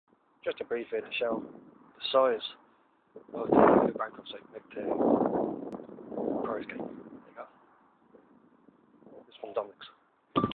new bancroft 74
one of two taken 27th september 2009, its a short one shot at the shops at st domincs near the church and clinic. the main reason was just to give a better sense of the scale of the block now as the vids on  the bridge dont do it justice.

its up to nine floors now.